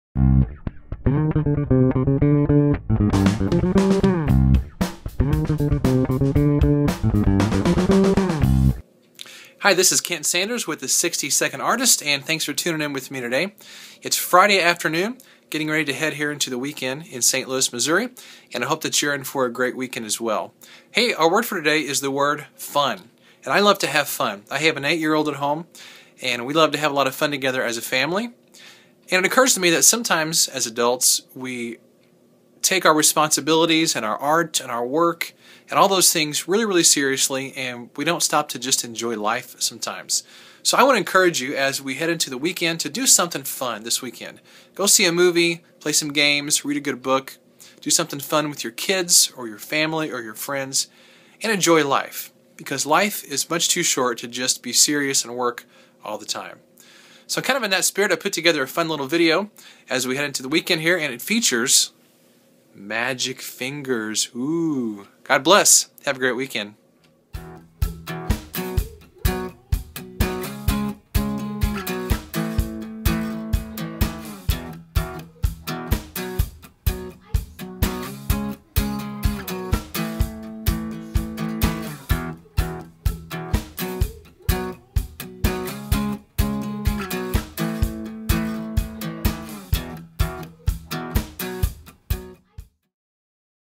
The 60-Second Artist: FUN (Coaching for Creatives)
We should should always take our work, art and responsibilities seriously, but sometime we take ourselves a little too seriously. Take some time to have fun with your family and friends! Life is meant to be enjoyed.